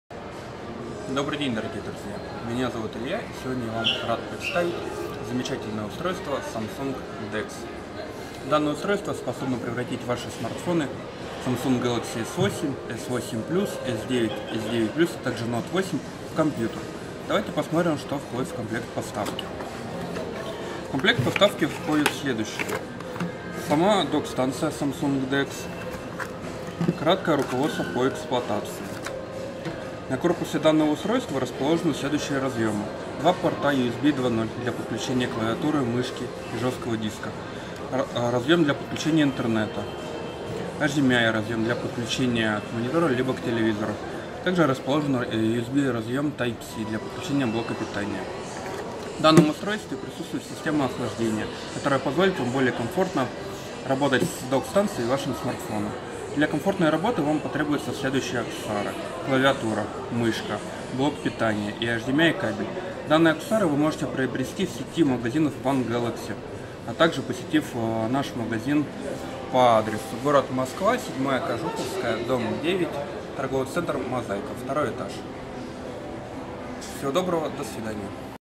Обзор Док-станция Samsung DeX Station!
Команда нашего магазина представляет вам обзор Док-станции Samsung DeX Station!
Адрес магазина: м.Дубровка (МЦК), 7-я Кожуховская улица, 9
ТРЦ Мозаика, 2-й этаж, Помещение №А214